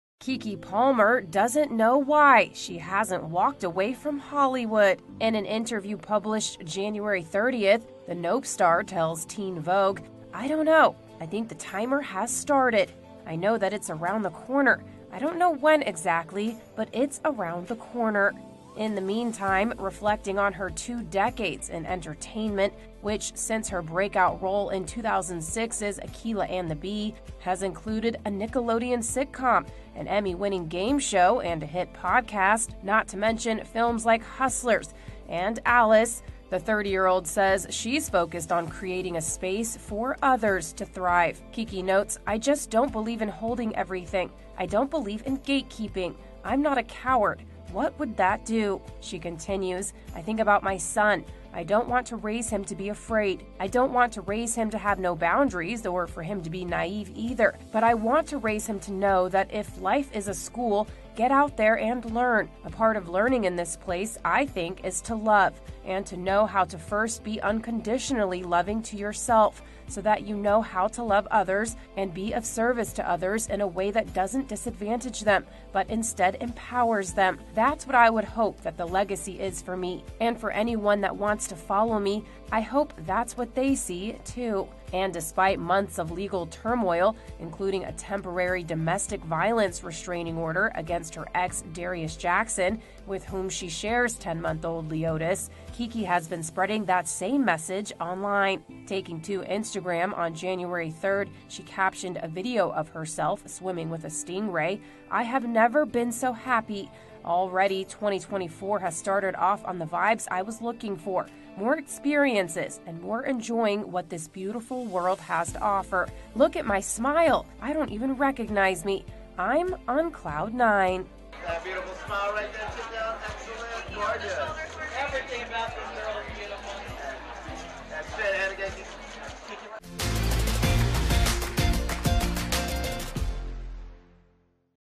Keke Palmer Says QUITTING Hollywood is “Around the Corner” | E! News
Keke Palmer says she might be ready to leave Hollywood amid months of legal turmoil, including a temporary domestic violence restraining order against her ex, Darius Jackson, with whom she shares 10-month-old son Leodis.